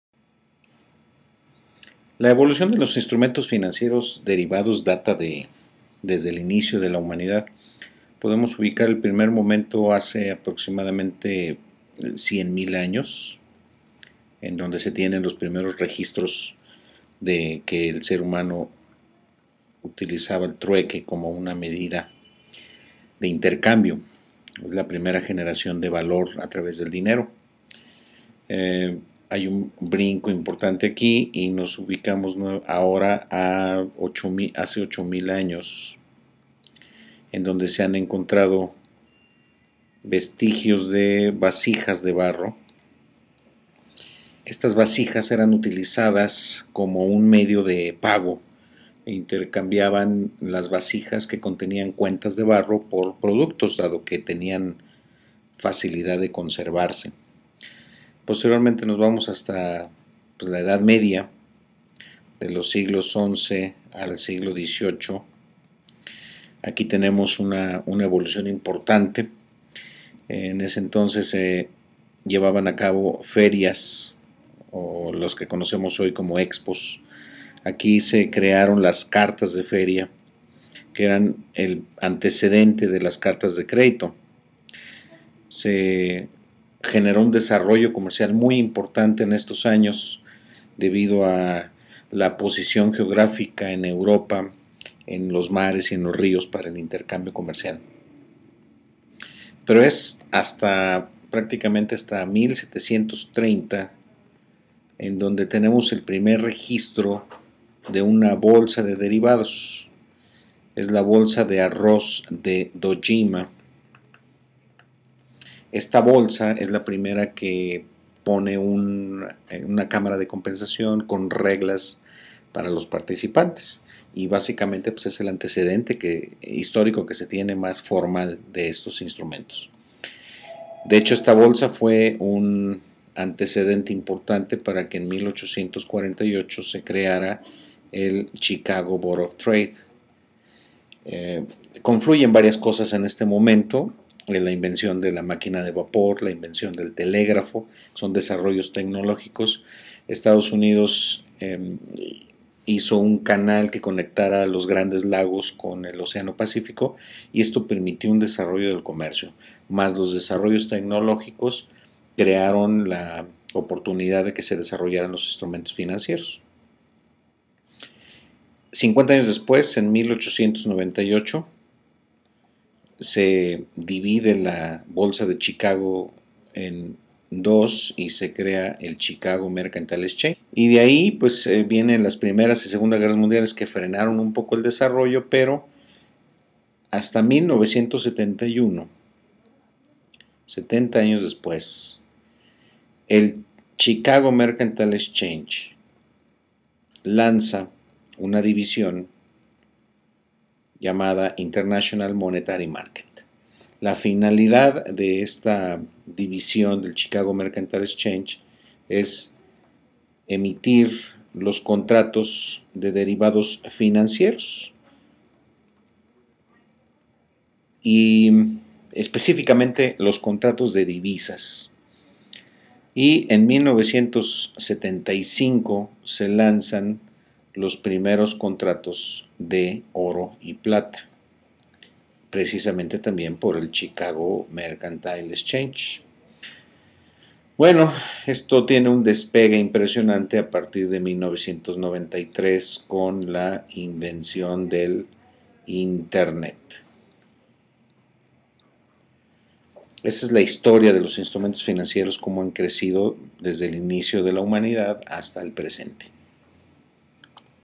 Historia de los Instrumentos financieros Derivados
Línea del tiempo, historia de los instrumentos derivados en 3 minutos.